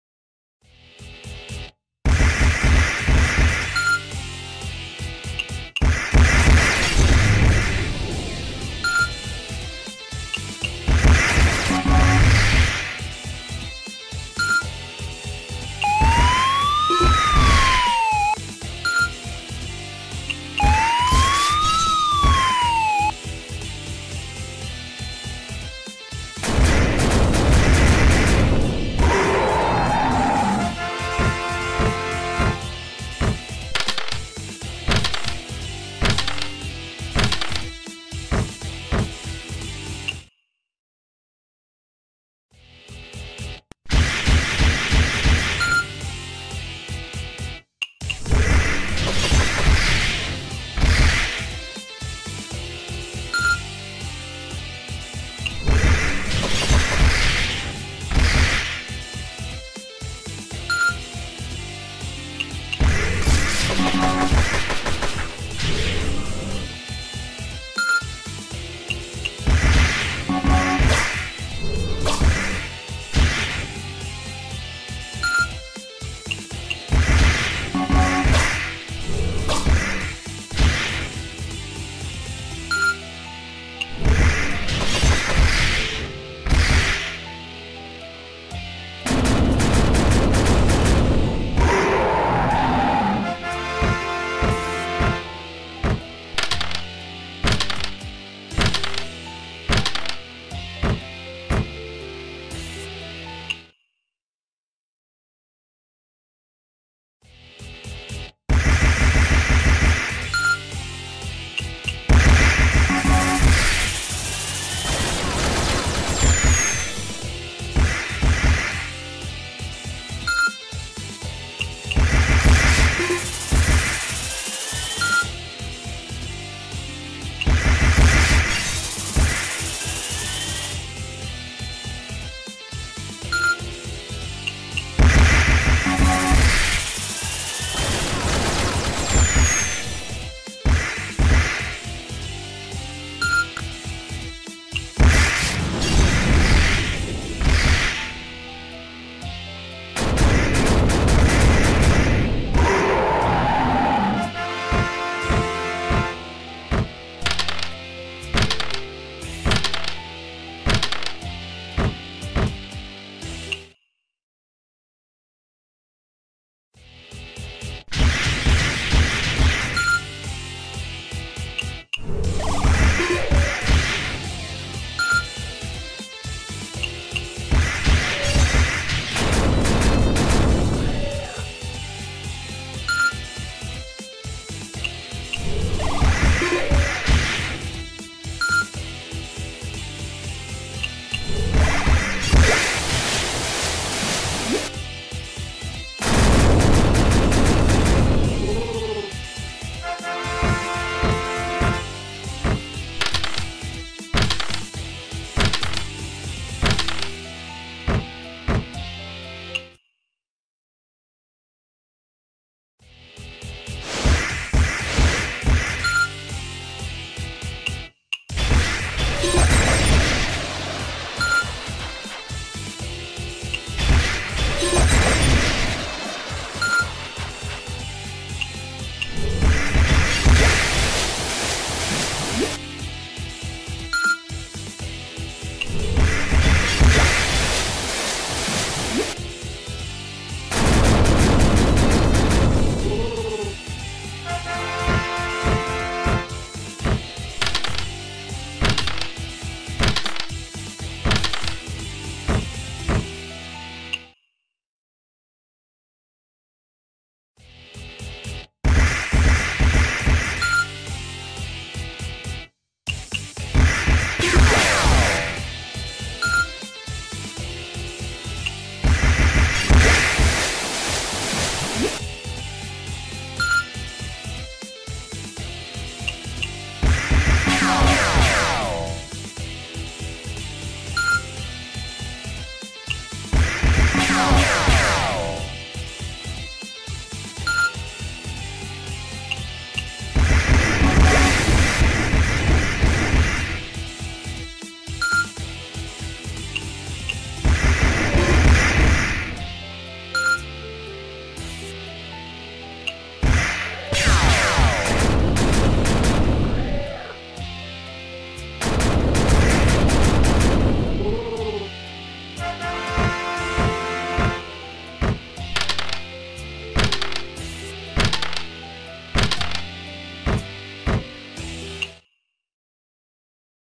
Fire Tyrannosaurus Hellfire Igniter + Triceratops Addition Water Tank - Dino Robot Battle Arena
00:00 - Fire Tyrannosaurus Hellfire Igniter
02:45 - Triceratops Addition Water Tank